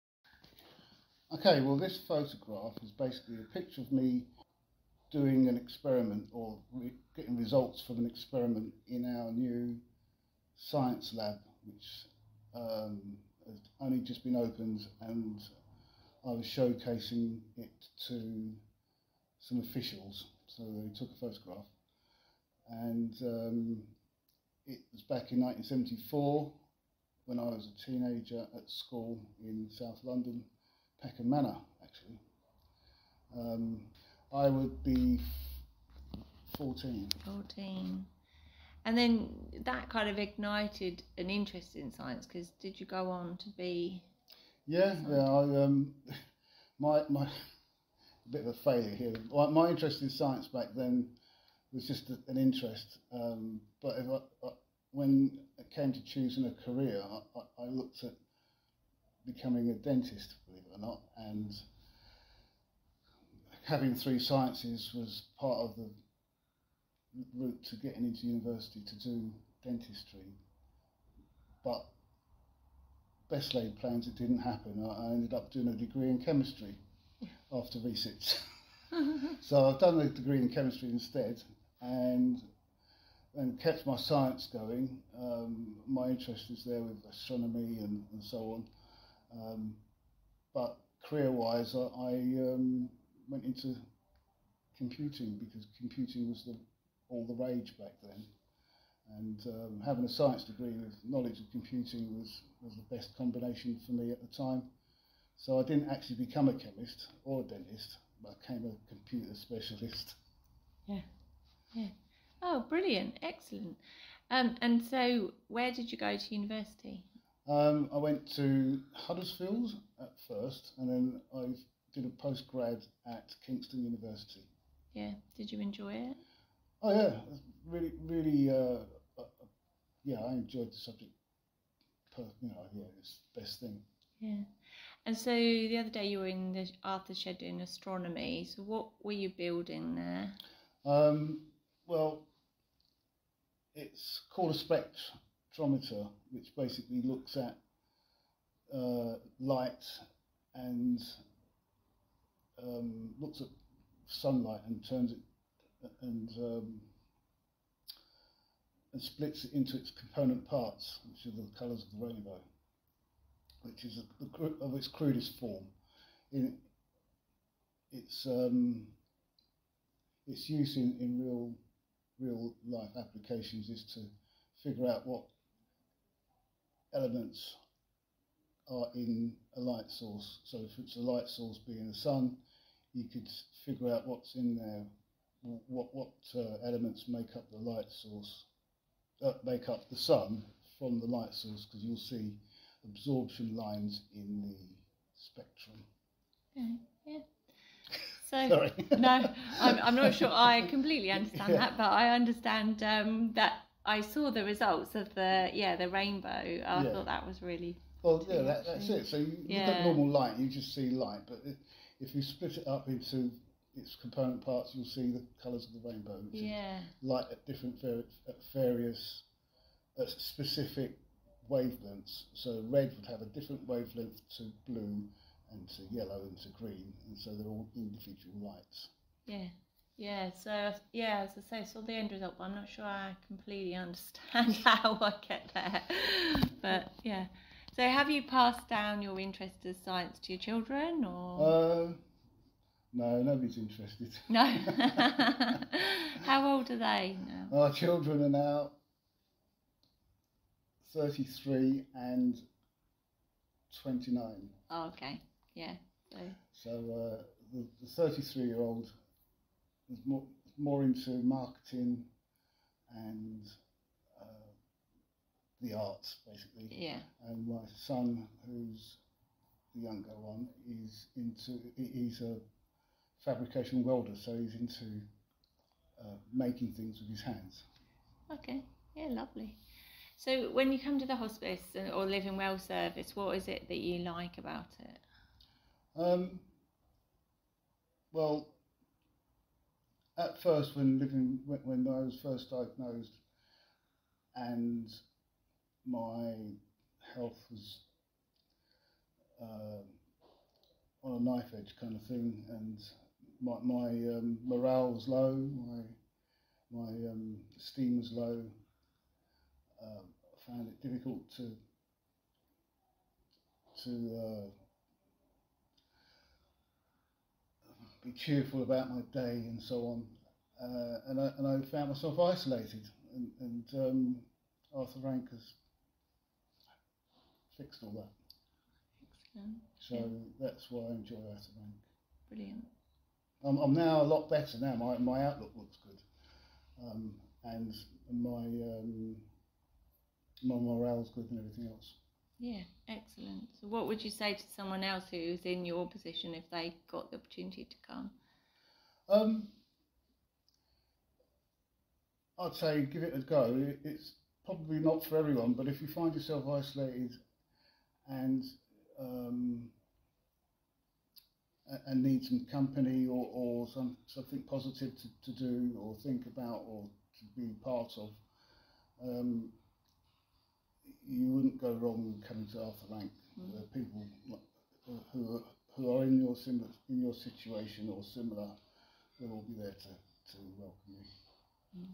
I'm dying to tell you how I'm Living Well. Stephen's Story
‘I’m Dying to Tell You How I’m Living Well’ gives people, who are living with a life limiting illness, the opportunity to reflect on life experiences, tell their story and collaborate in a legacy project, resulting in individual photobooks.

But most importantly, it is about a celebration of life. It is about living. This is not a project about grief or loss but about seizing the opportunities for interactions and connections that we have today – right now.

An audio of participants talking about their photographs was captured and can be heard by following the QR codes as ‘living books’.

CHAPTERS

00:00 Tell me about your photograph
04.41 What do you like about the Living Well Service?
05:43 What would you say to someone else invited to the Living Well Service?